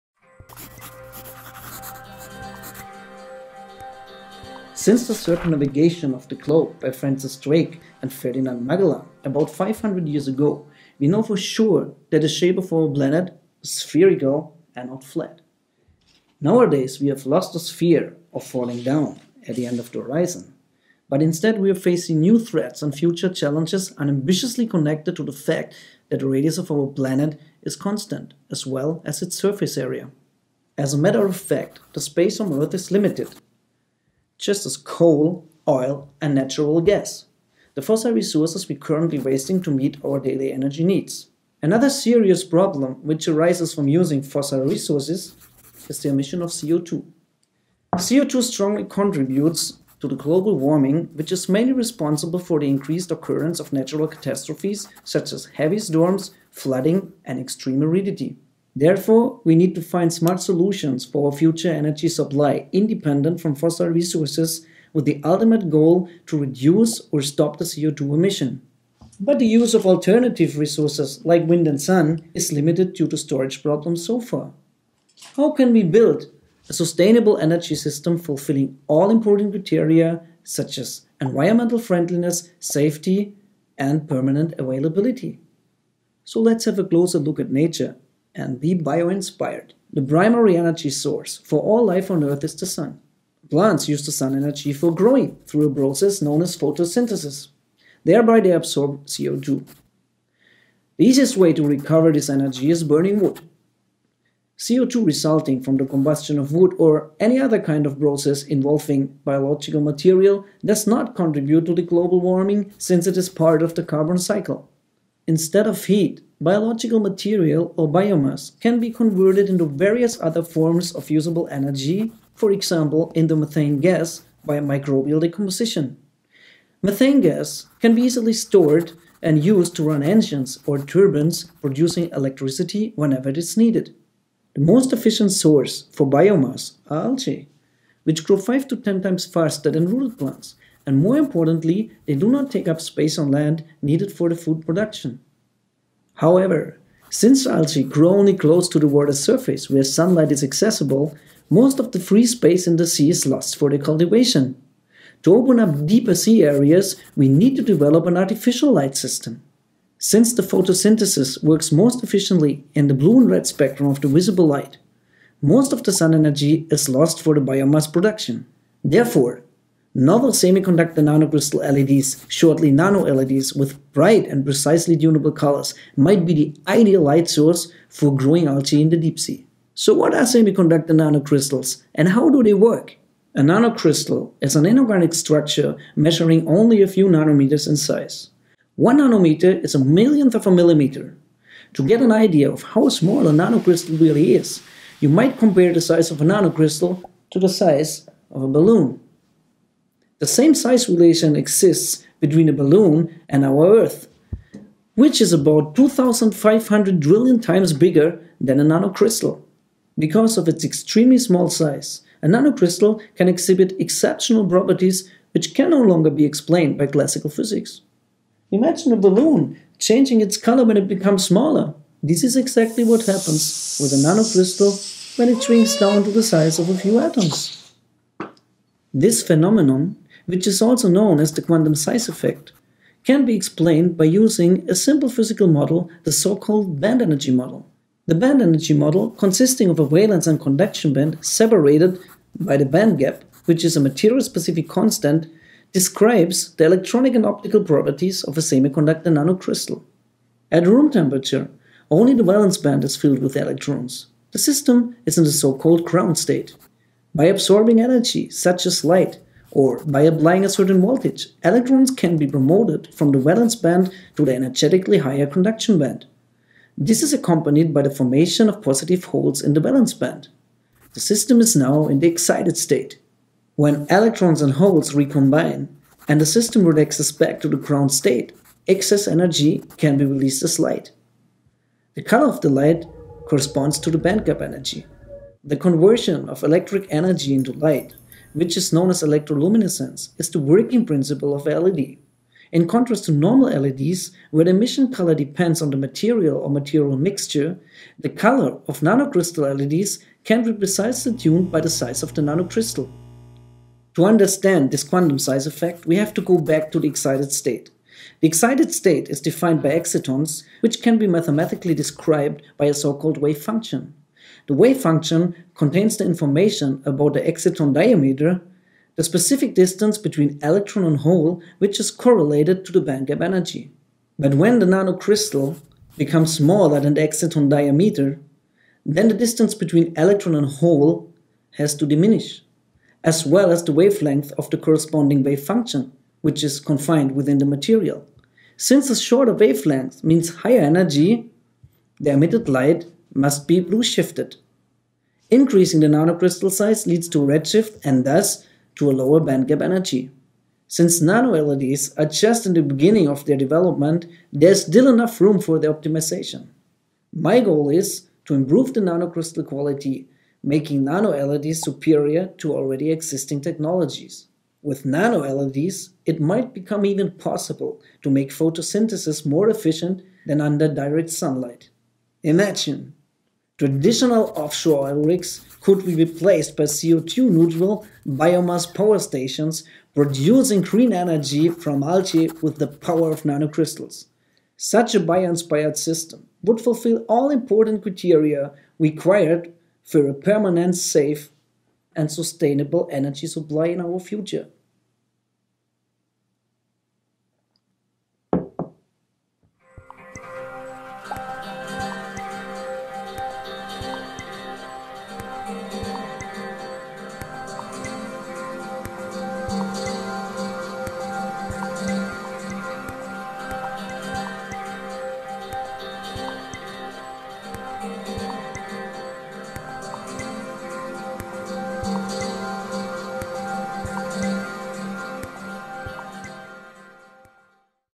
Can nanocrystals stop the climate change ?
Fossil resources are limited and their CO2 emission strongly contributes to the global warming which is mainly responsible for the increasing appearance of natural catastrophes. Renewable biomass which can be converted into various forms of usable energy such as methane gas, might be a promising alternative to coal, oil and natural gas. CO2 resulting from biomass does not contribute to the global warming because it is part of the carbon cycle. Algae which grow 5 to 10 times faster than rooted plants on land, are the most efficient source for biomass. However, their cultivation in deeper sea areas requires an artificial light system which should be robust, energy-saving and emit in the blue and red spectrum of the light where photosynthesis works most efficiently. The emission color of novel Nanocrystal LEDs or NanoLEDs can be precisely tuned over the whole visible range by just changing the nanocrystal size, an optical phenomenon known as quantum-size effect. Using optimized NanoLEDs might enable plants to grow even faster than under direct sunlight. In the future, traditional off-shore oil rigs could be replaced by CO2 neutral biomass power stations producing green energy from algae and NanoLEDs powered by the abundant energy of wind and sun.